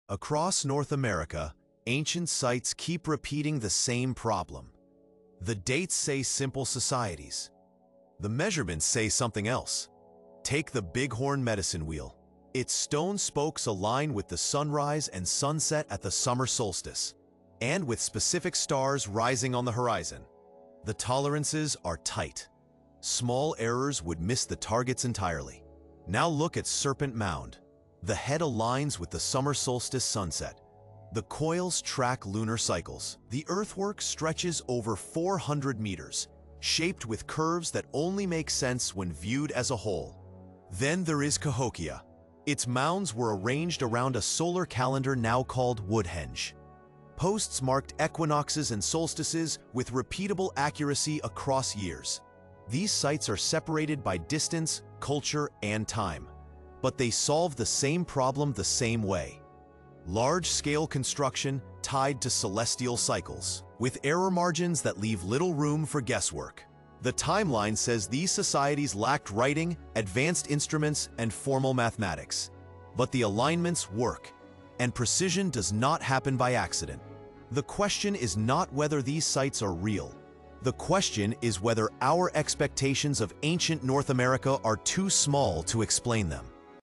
What Ancient Americans Knew About The Sky
Ancient sites across North America show precise alignments to the sun, moon, and stars. The dates are accepted. The measurements are repeatable. But the level of accuracy does not sit comfortably with the standard timeline.

This short compares three sites using only dates, targets, and tolerances. No shared culture claims. No lost civilization stories. Just the recurring pattern that keeps raising the same quiet question.